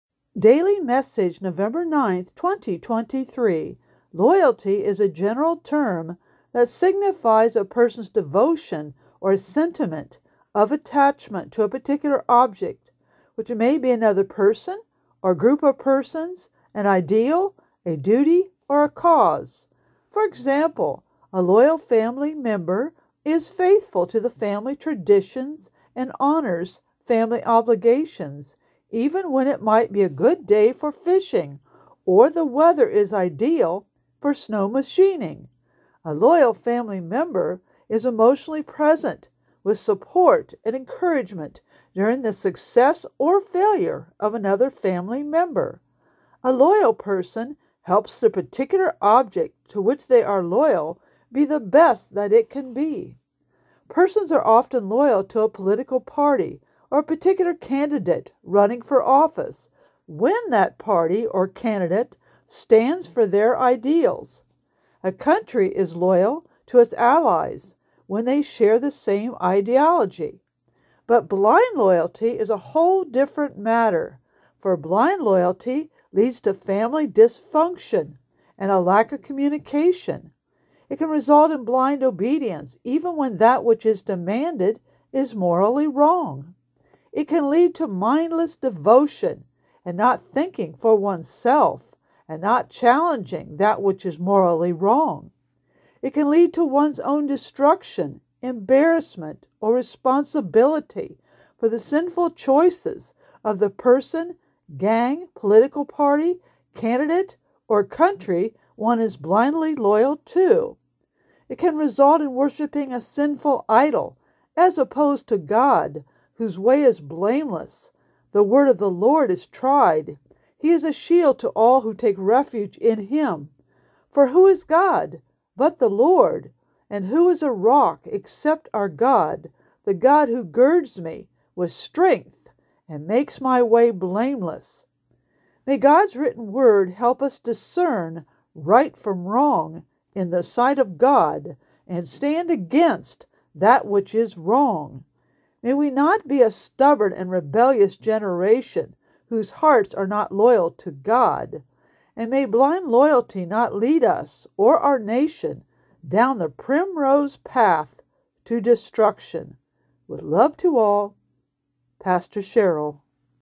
Daily Message 11092023 - Loyalty Versus Blind Loyalty
THEME: Beware of blind loyalty for it can lead one down the primrose path to destruction. It can result in worshiping a sinful idol as opposed to God whose way is blameless and whose word is tried and true.  SCRIPTURAL FOUNDATION: Psalm 18:30-32, Mark 12:30-31.